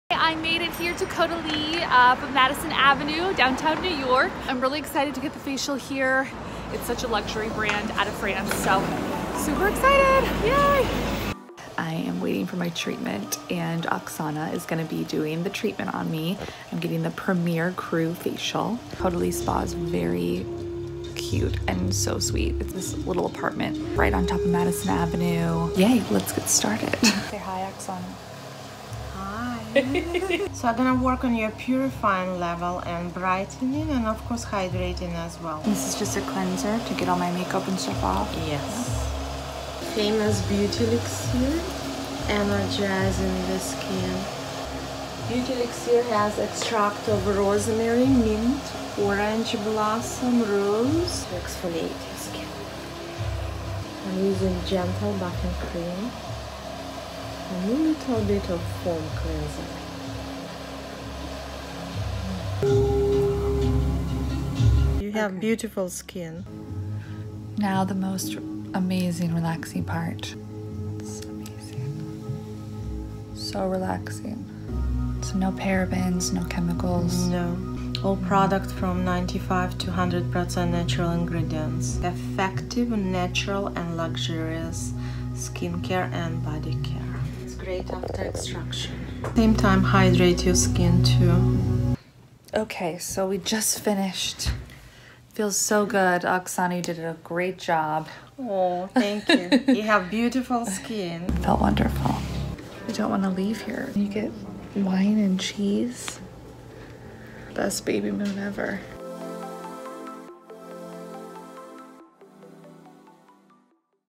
Brit Unscripted: Caudalie
Brit visits the Caudalie Spa in New York City. The company is based out of Bordeaux, France where its vine and grape extracts are used for its patents that come from the vineyards. There are NO  Parabens, Phenoxyethanol, Mineral oils, Paraffins, and Phthalates used in any of its products and I really loved the experience at the spa in NYC!